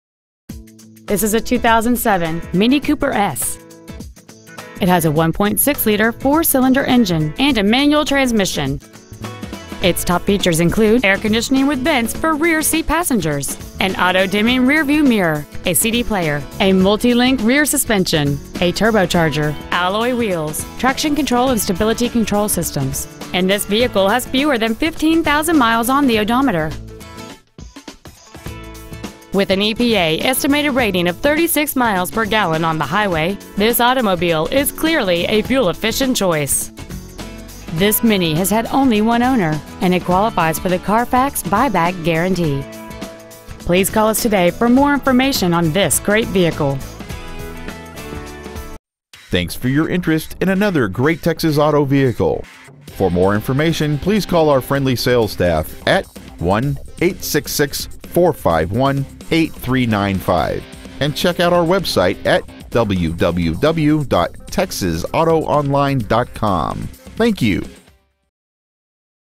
2007 MINI Cooper S Austin TX
We are proud to present this 2007 MINI Cooper S .

 Year : 2007

 Make : MINI

 Model : Cooper S

 Engine : 1.6L OHC turbocharged 16-valve I4 engine

 Trans . : Manual

 Exterior : White

 Miles : 14,719

 Interior : Black

 Texas Auto

 5531 Mitchelldale St.

 Used 2007 MINI Cooper S Houston TX